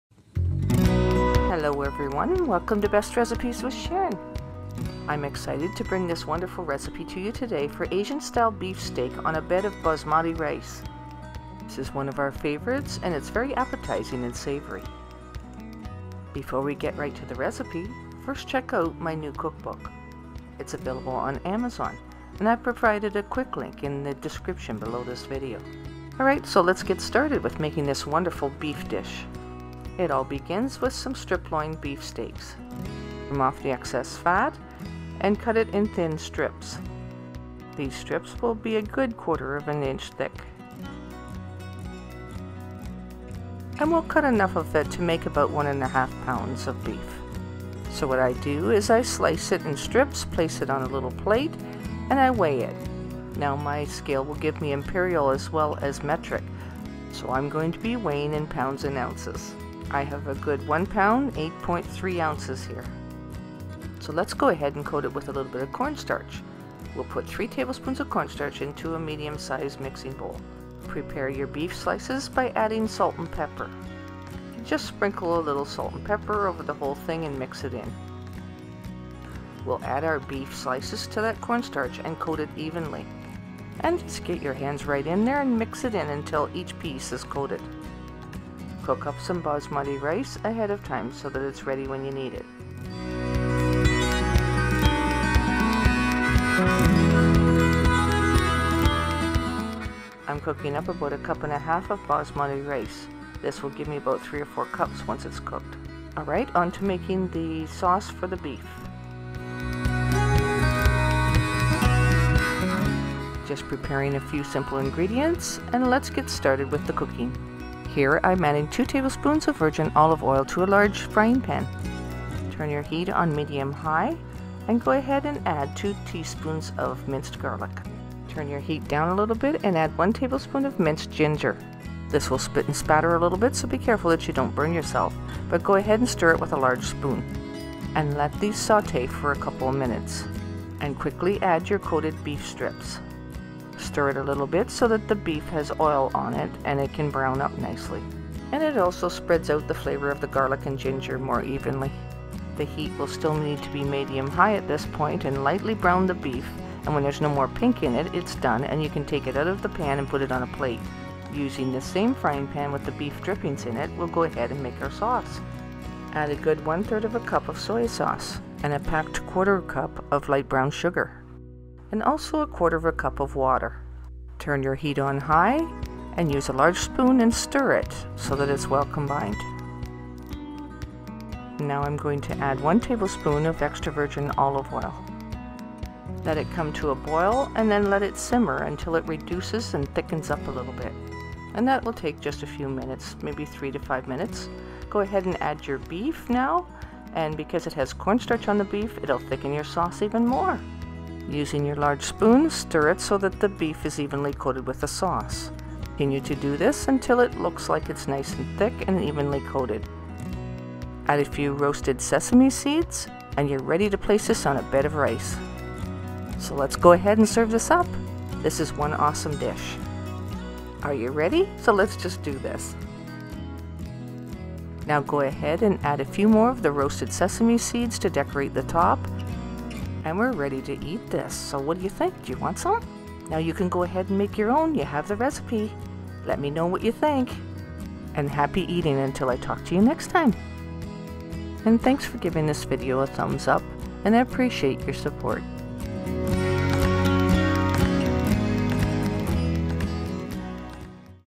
ASIAN MONGOLIAN STYLE BEEF STEAK
Tasty and Tender Beef Dish that satisfies the craving for a Hearty Meal that is quick to make too!

PLEASE CLICK THE QUICK LINK BELOW TO PURCHASE MY COOKBOOK;